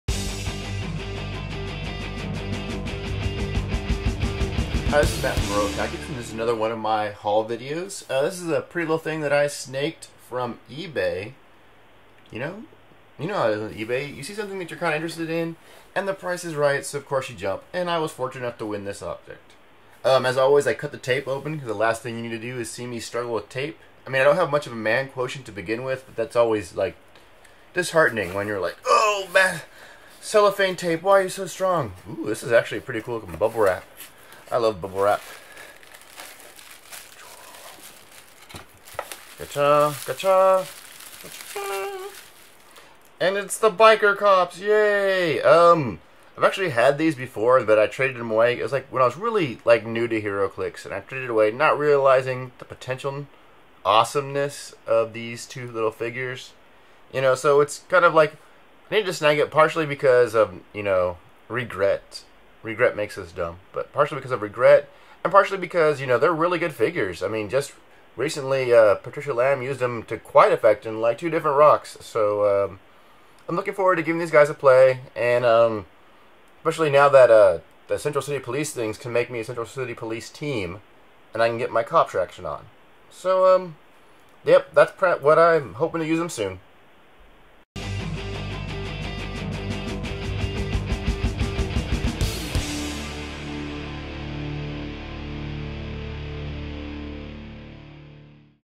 HeroClix Haul #2
Matt opens up a package from eBay!